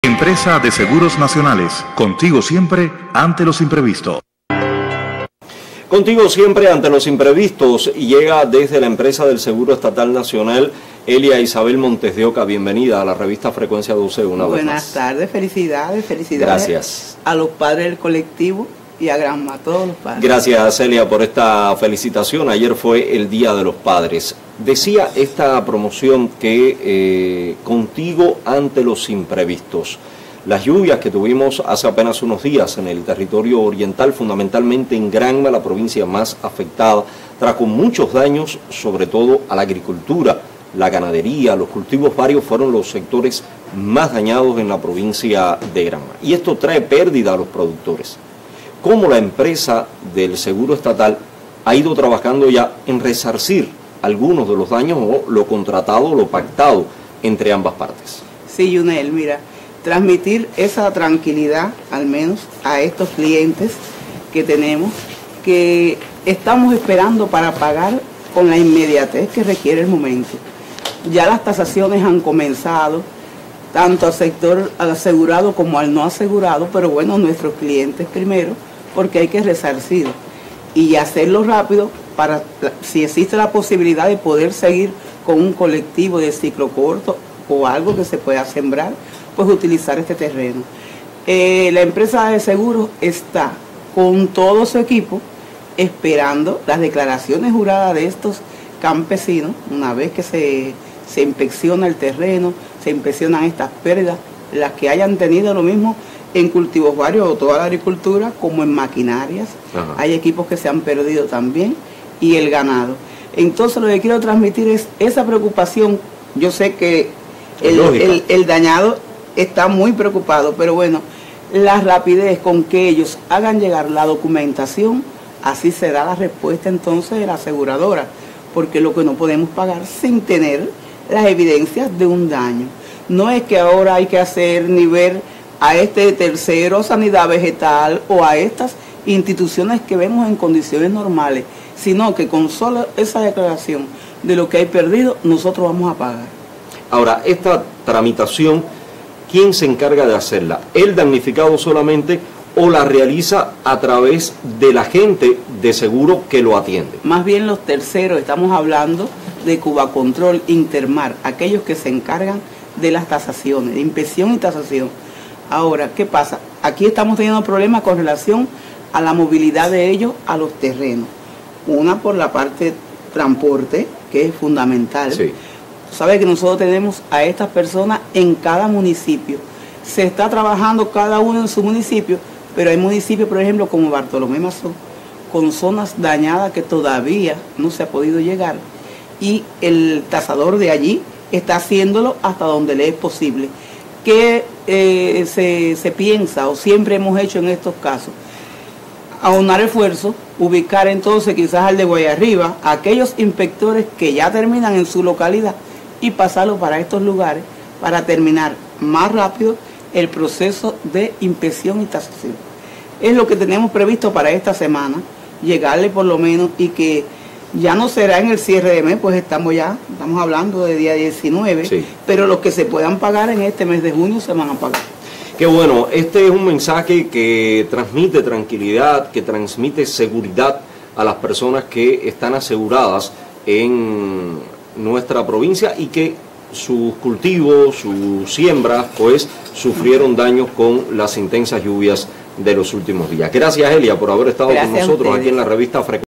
La Empresa Provincial de Seguros de Granma resarcirá daños a productores afectados por las lluvias
La Empresa Provincial de Seguros Estatal de Granma resarcirá a productores cuyas plantaciones se vieron dañadas por las Intensas lluvias en Granma, aseguró en Frecuencia 12 Elia Isabel Monte Oscas Veranes, especialista de la ESEN